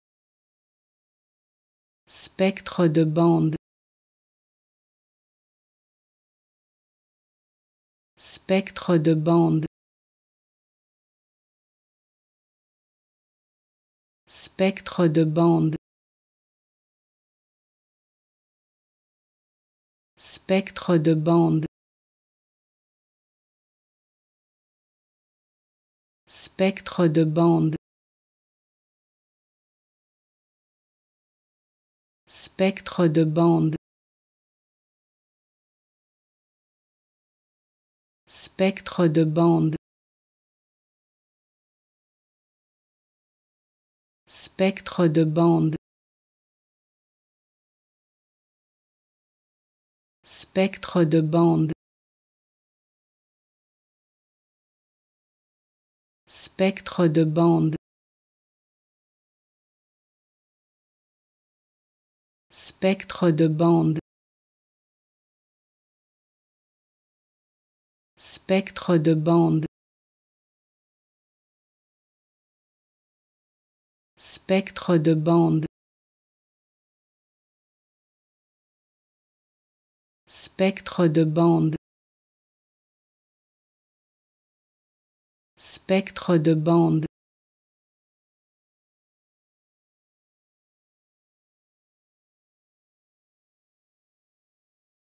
How to Pronounce ''Spectre de bandes'' (Band spectrum) Correctly in French
Learn how to say and properly pronounce ''Spectre de bandes'' in French with this free pronunciation tutorial.